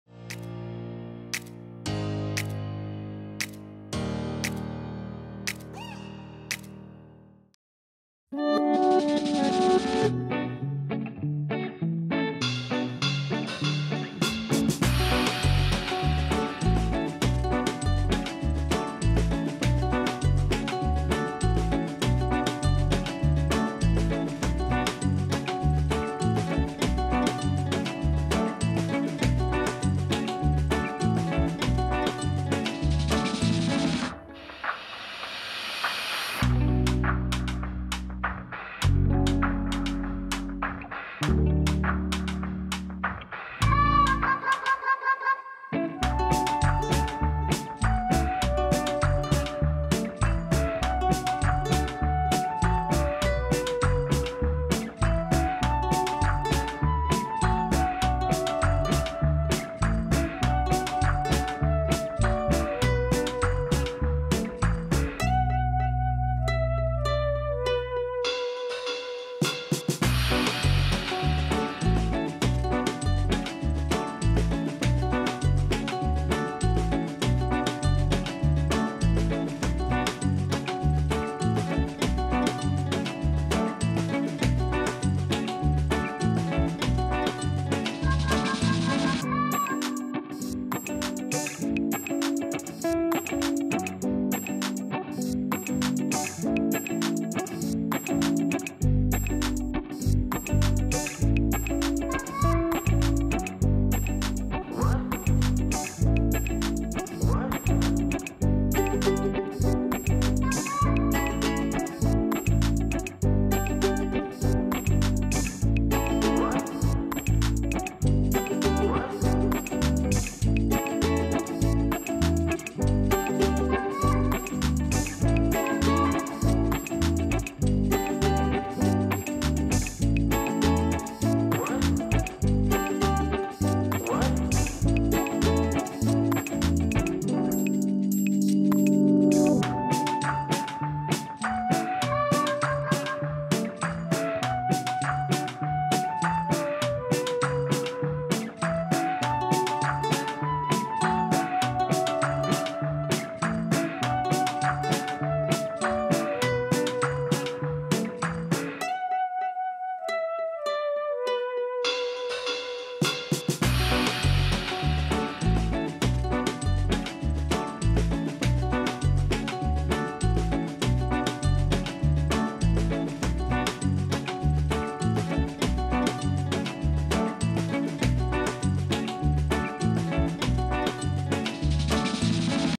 Turkey Coloring Pages | Fun And Learn Turkey Coloring Videos for Kids
Get ready to gobble up some creative fun with our delightful Turkey Coloring Videos!

🦃🎨 Whether you're a seasoned artist or just starting out, join us in bringing these festive birds to life with a burst of colorful imagination.
🌈✨ Press play and let the artistic journey begin!